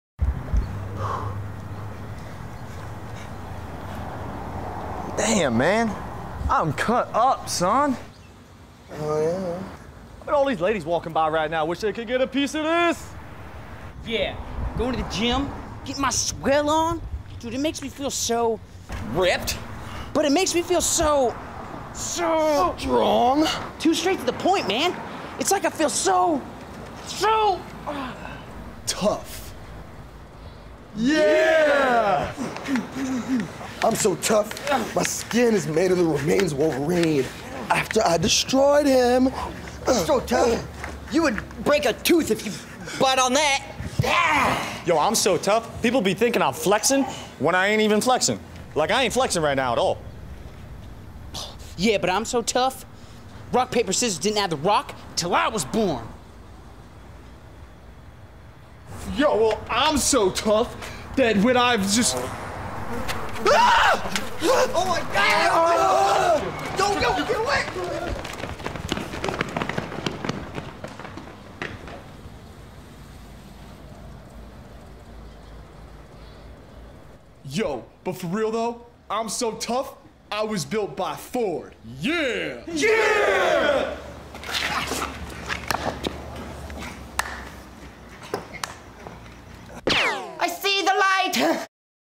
Tough Guys
The boys decide to start working out.